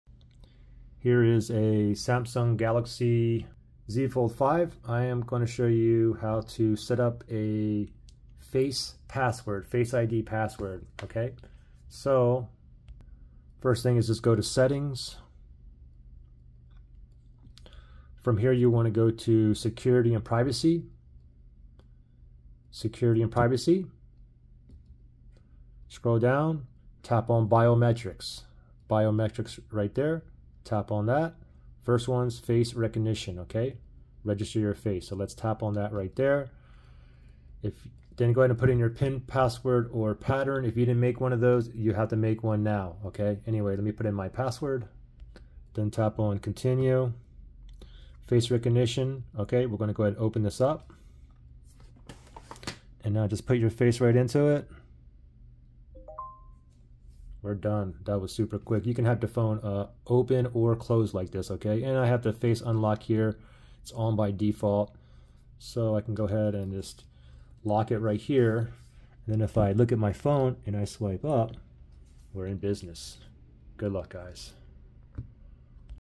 Galaxy Z Fold 5: how to Setup Face ID Password
I show you how to setup/add a Face ID  password (face recognition) on the Samsung Galaxy Z Fold 5 smartphone.

SAMSUNG Galaxy Z Fold 5 Unlocked smartphone